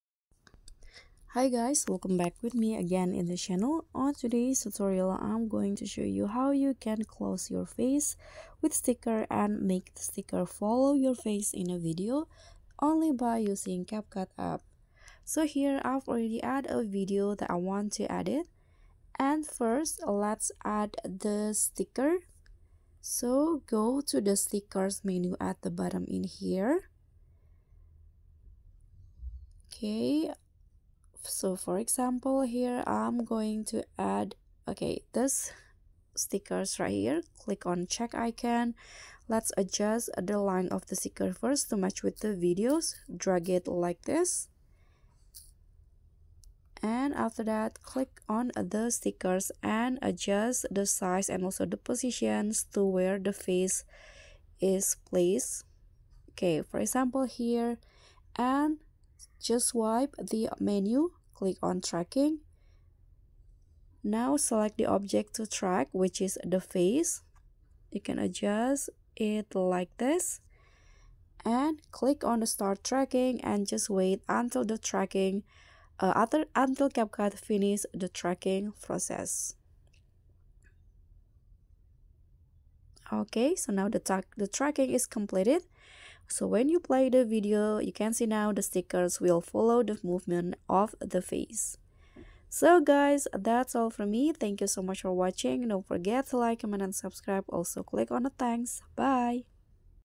[NEW UPDATE] How to Cover Your Face with Sticker and Make It Follow Your Face in Videos Using CapCut
This is how you can cover your face with stickers on CapCut